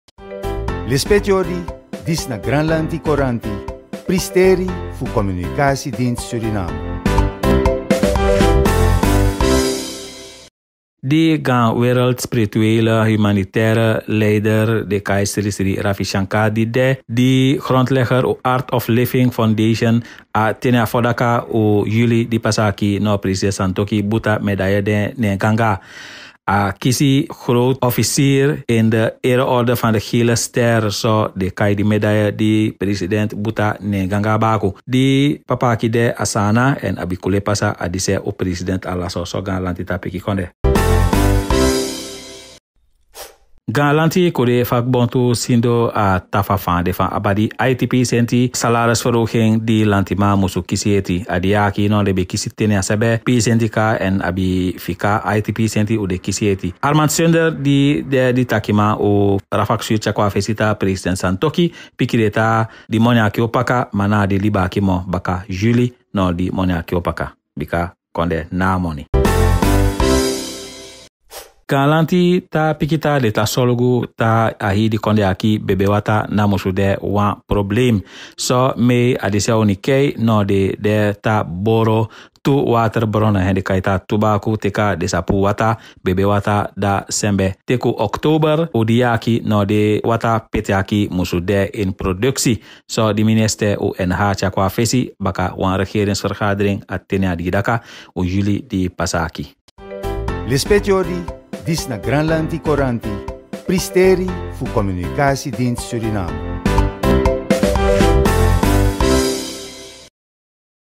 𝔾ℝ𝔸ℕ𝕃𝔸ℕ𝕋𝕀 𝕂𝕆ℝ𝔸ℕ𝕋𝕀 (SARAMACCAANS) DD 15 JULI 2022
De onderwerpen in deze aflevering zijn:
- President Santokhi honoreert SriSri Ravi Shankar met hoge ondersteuning
-Regering en vakbeweging in overeenstemming over uitbetaling 8% loonaanpassing
-Regering geeft prioriteit aan duurzame drinkwatervoorziening